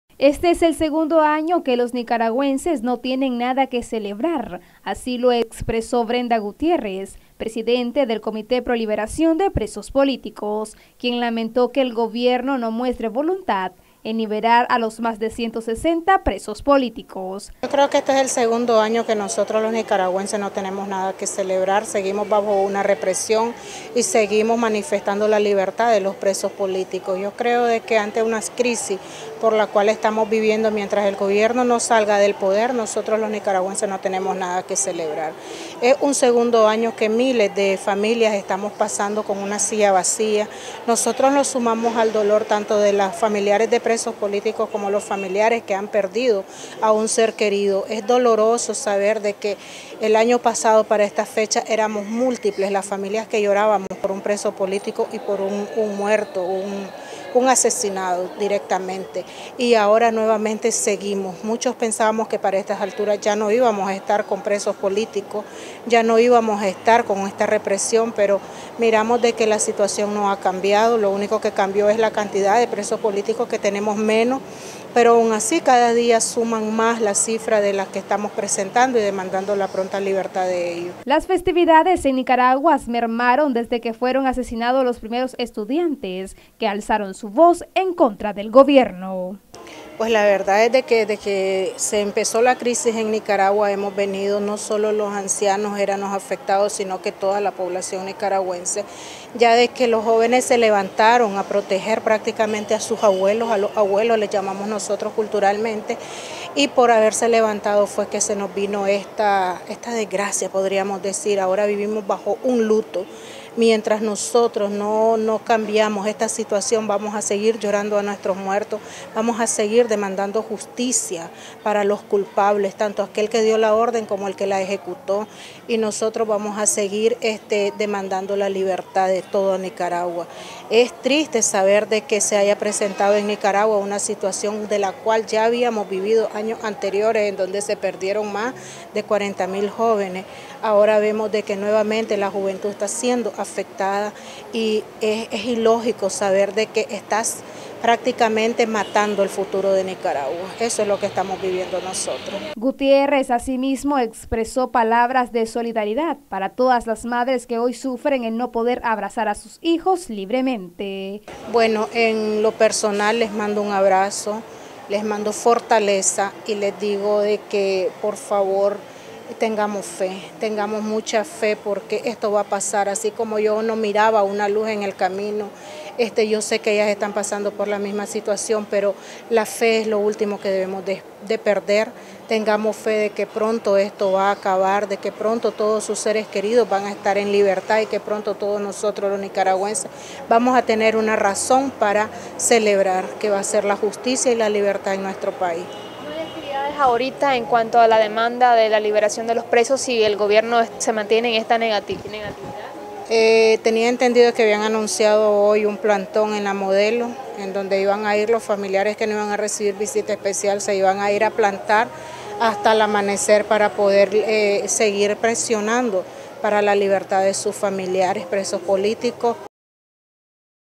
EN NICARAGUA NO HUBO NADA QUE CELEBRAR
Gutiérrez: Los nicaraguenses no tenemos nada que celebrar.